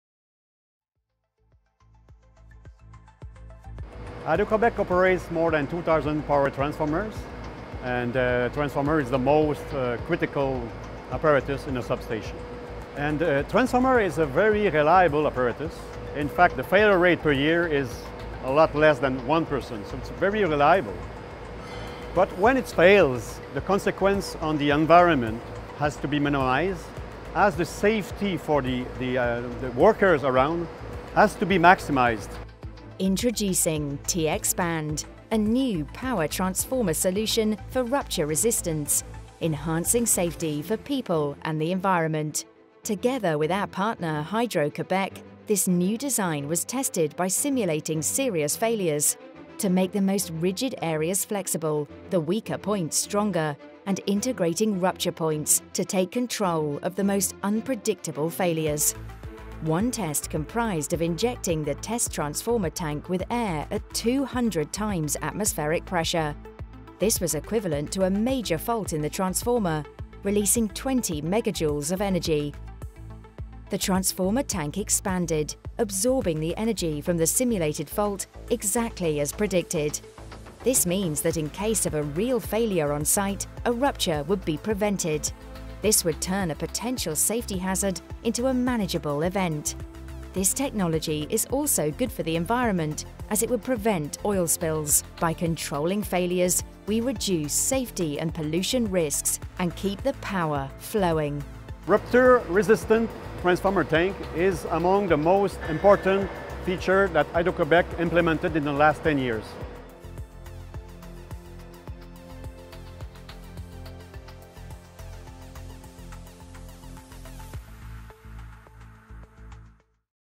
TXpand™ – Transformer solution enhancing safety for people and the environment
TXpand™ is a rupture resistant transformer solution. Transformers utilizing this technology can literally expand, absorbing energy from faults and enhancing safety for people and the environment.